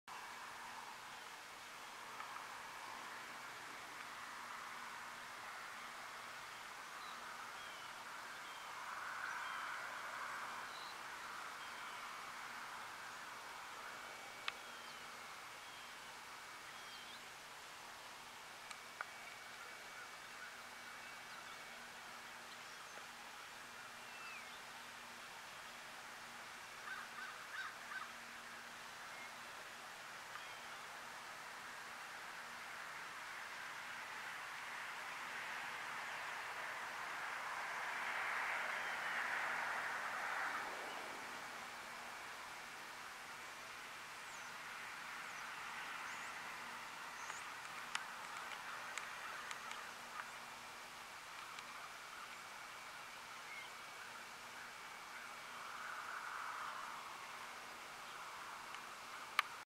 Canon Vixia HF G30 Sunset Shot In Black & White
Here's another West Branch, Michigan sunset.